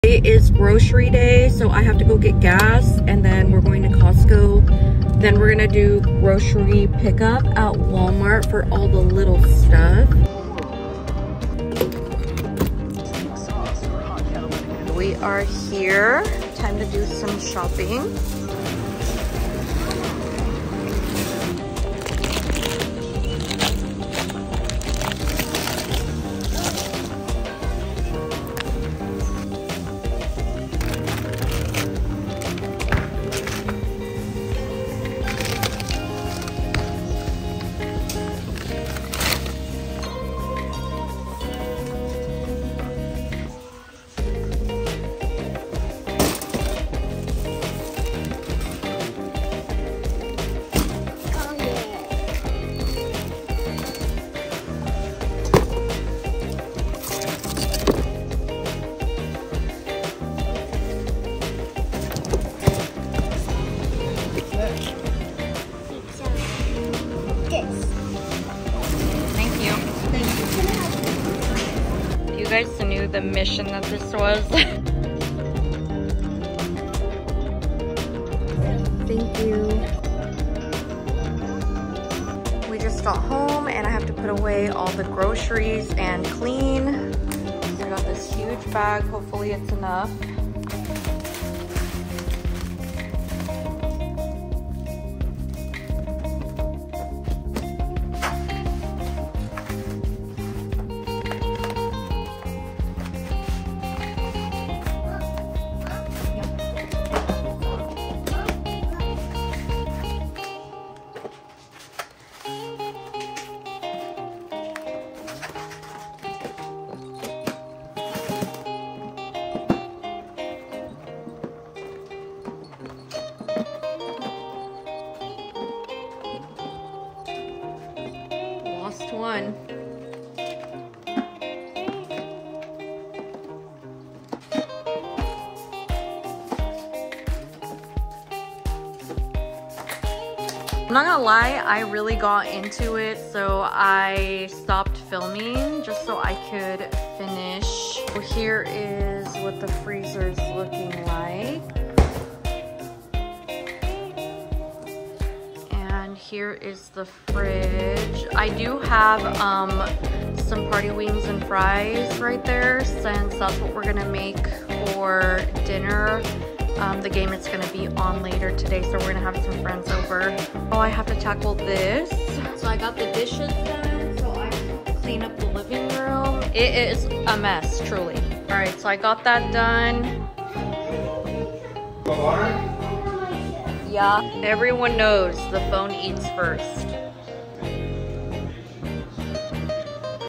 Day in the life of a Latina Stay at Home Mom Vlog | Grocery Day: Costco + Walmart Pickup
I hope you enjoy this vlog sharing my day!